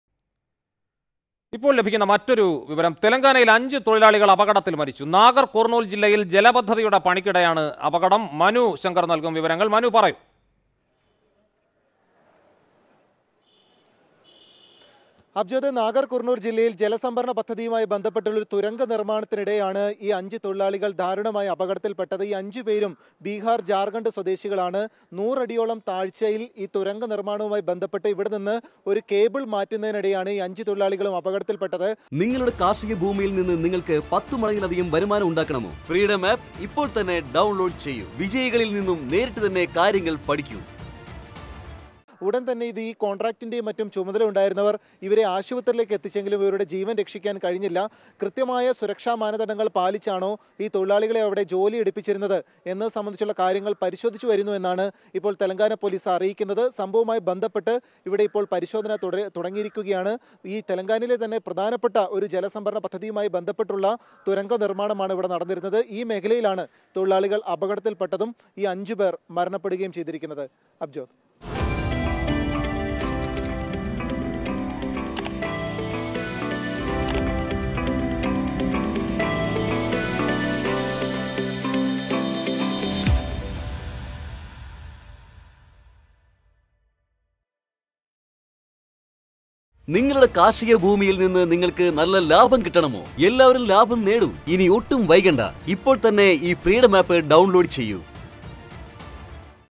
തെലങ്കാനയിൽ നിർമ്മാണ ജോലിക്കിടെ അപകടം; അഞ്ച് തൊഴിലാളികൾ മരിച്ചു
തെലങ്കാനയിൽ ജലപദ്ധതിയുടെ നിർമ്മാണ ജോലികൾക്കിടെ അപകടം; അഞ്ച് തൊഴിലാളികൾ മരിച്ചു; ജലപദ്ധതിയുമായി ബന്ധപ്പെട്ട തുരങ്ക നിർമ്മാണത്തിനിടെയായിരുന്നു അപകടം